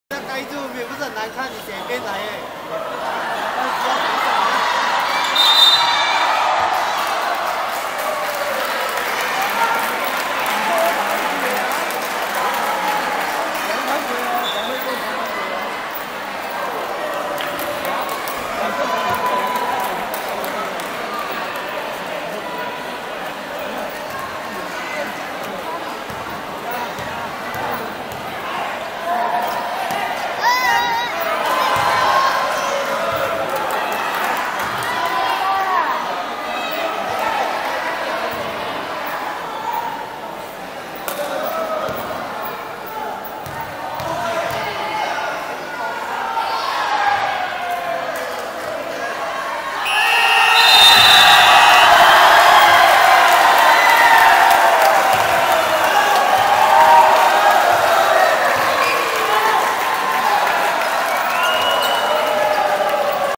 BATANG MELAKA BOLA KERANJANG PIALA BELIA 2014 (18)
05.hb.Sep..PERLAWANAN BOLA KERANJANG PIALA BELIA 2014 , ANJURAN PERSATUAN BELIA BATANG MELAKA . P/S:  KG.BARU ON LOK VS SEKOLAH TINGGI KATOLIK.